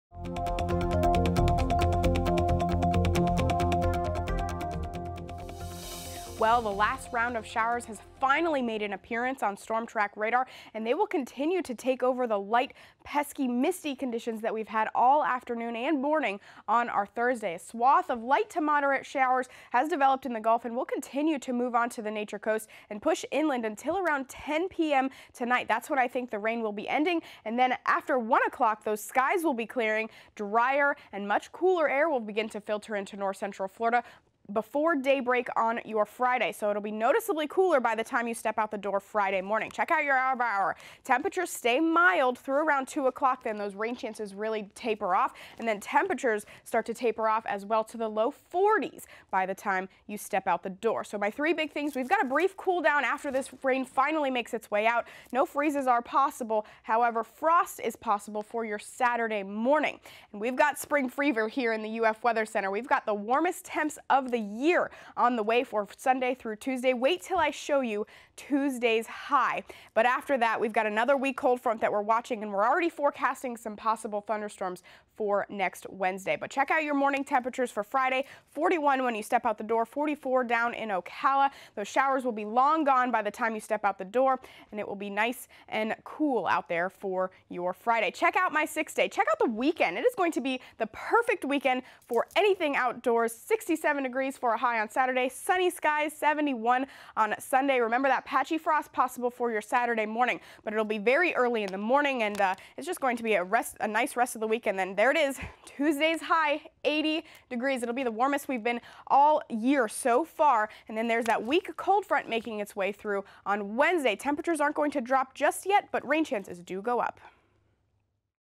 Rain Moving Out, Nicer Weekend Ahead
UF Forecaster Amanda Holly is tracking the back edge of the rain, a nicer weekend ahead, and a HUGE warm up next week!  Who's ready for 80 DEGREES?